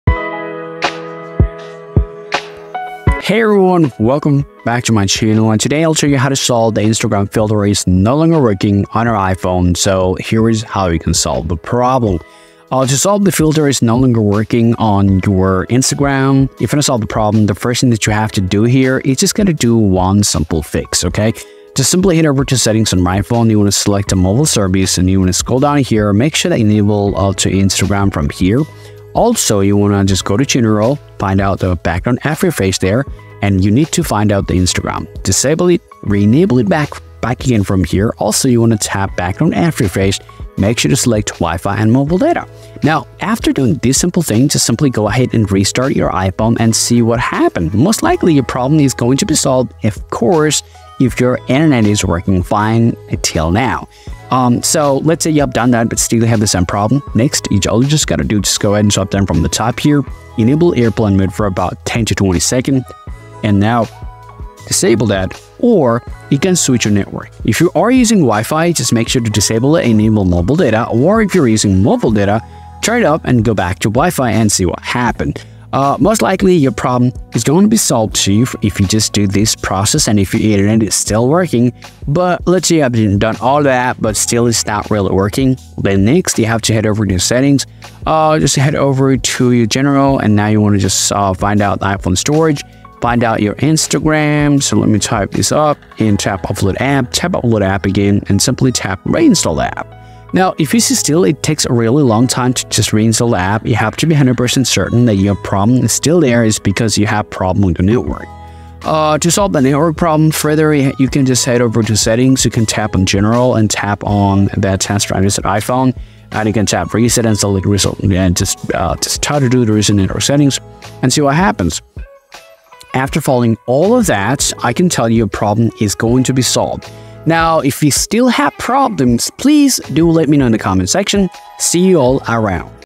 How to Fix Instagram Filter Not Working Problem on iPhone | IG Filters Not Showing Up
Is your Instagram filter not working on your iPhone? If your Instagram story filter not working, no worries. I'm here with a solution video on how to solve the Instagram story filter problem. In this video, I will show you how to fix the Instagram story filter problem and the right process for solving the Instagram story effect problem.

Watch the video to discover how to fix the Instagram story filter. Follow the instructions step by step carefully to overcome the Instagram filter not showing issue and learn the method to fix the Instagram story effect not show issue.

00:01- Video intro
00:12- The Solution
02:06- Video ending